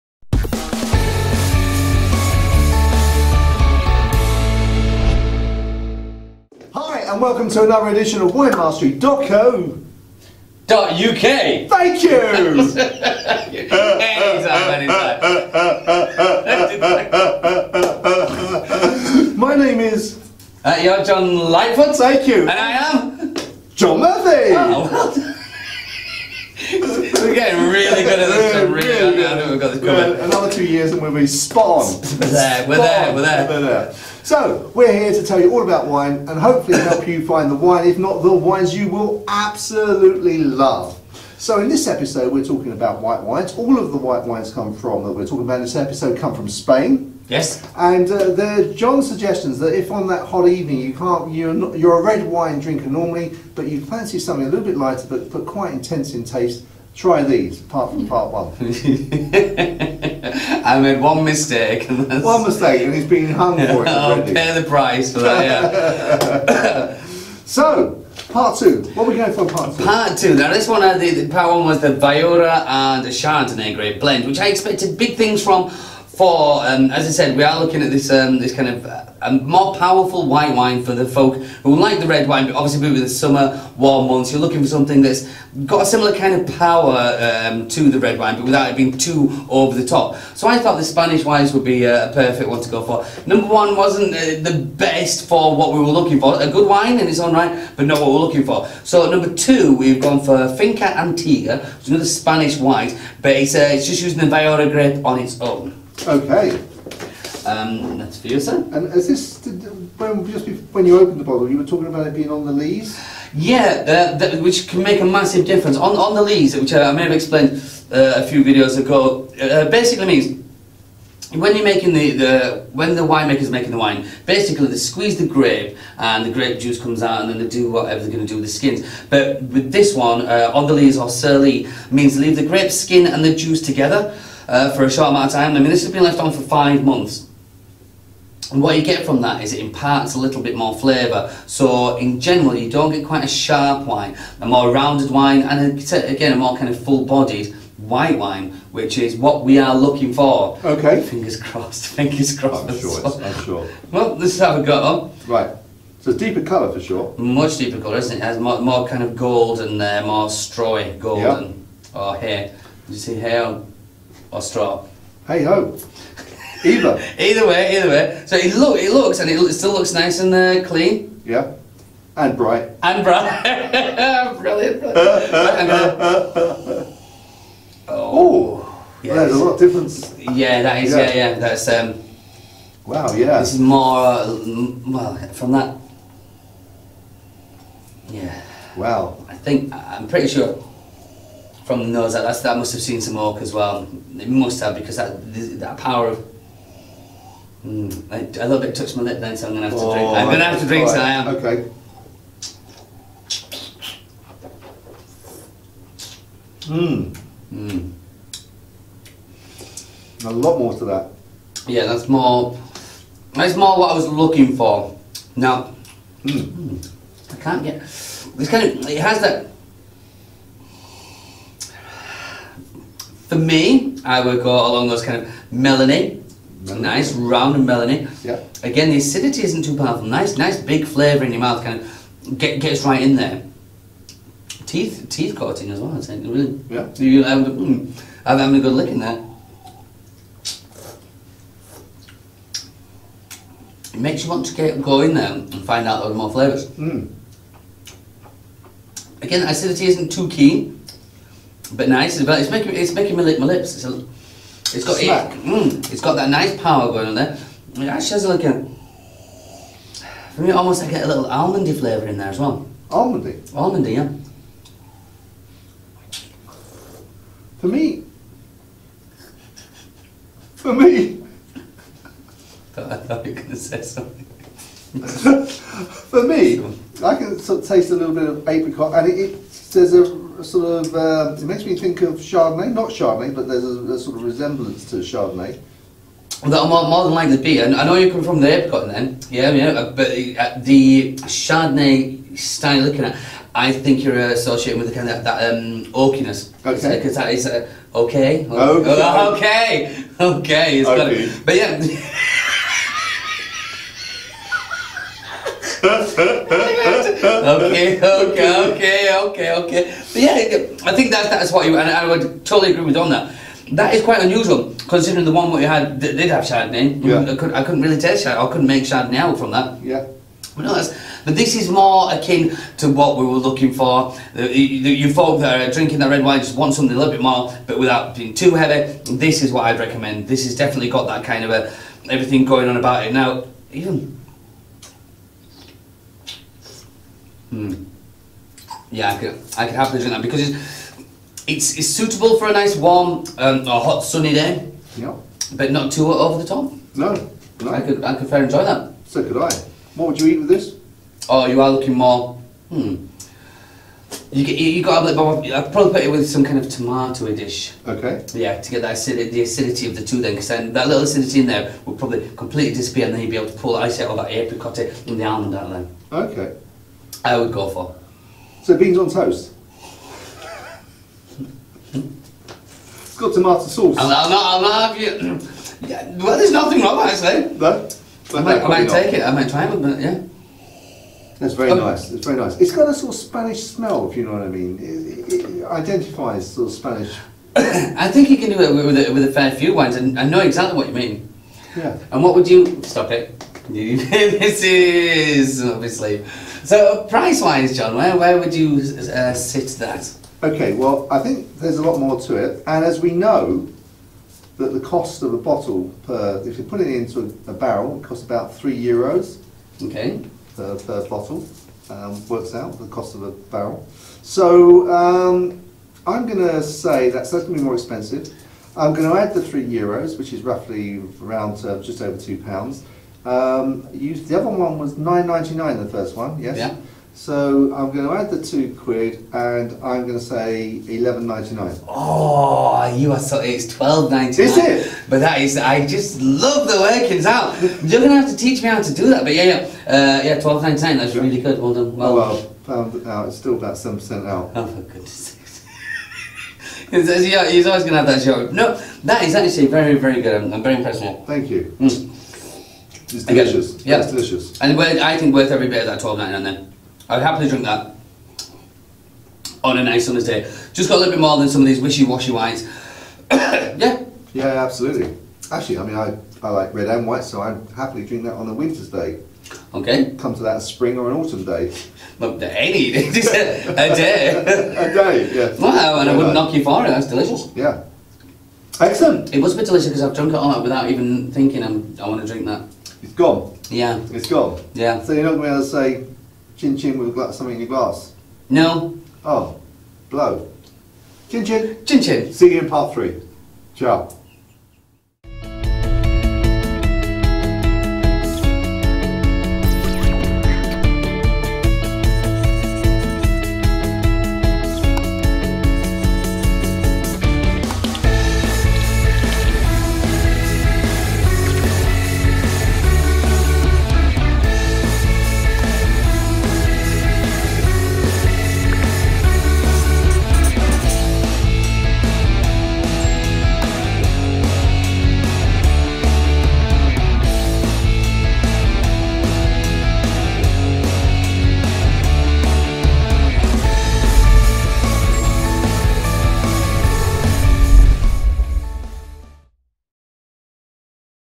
Tasting a White Viura as a alternative for the Red Wine Drinker (Episode 55 Part 2)
Now on  to our second alternative White Wine for the Red Wine drinker.

In this part of the episode, we taste another wine made from the Viura grape, this time from the La Mancha region of Spain.

This wine has spent "5 months over lees"  and is produced by Finca Antigua

and

Should you wish to taste this wine for yourselves it is available